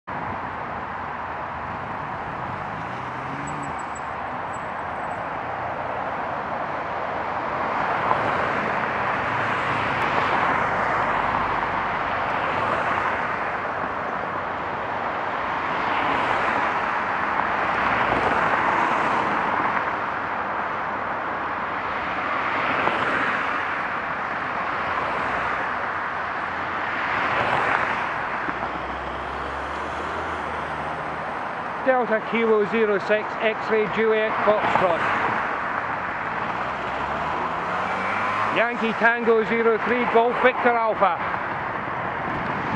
DK06XJF YT03GVA pure ignorance
Overtake someone waiting to turn left, now that's real shit driving. DK06XJF doesn't even manage to indicate he's turning left. No, I'm not stopped here other than waiting for a gap to turn left.

Monday 22nd May 2017 approx 08:20